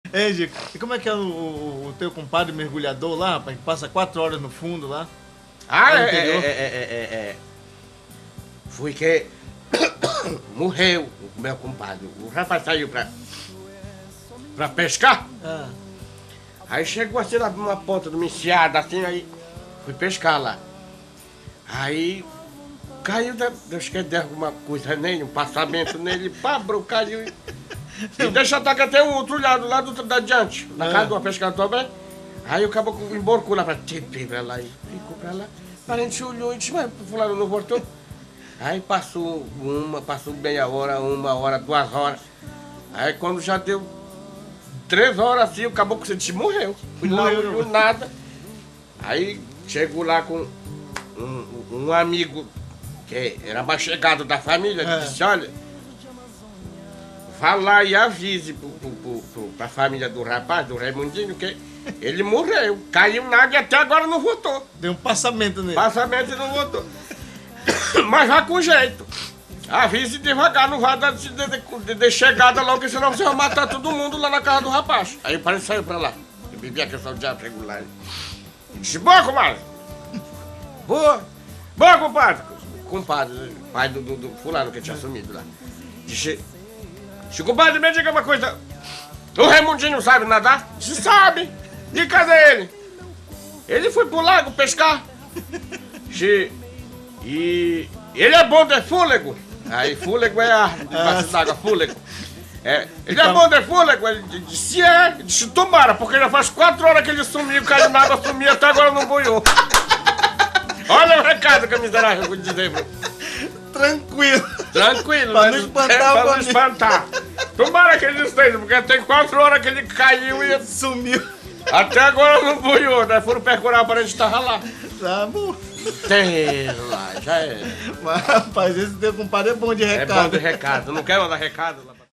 Rigico Piada Bom Recado
Esta é uma historia de um compadre muito bom de recado.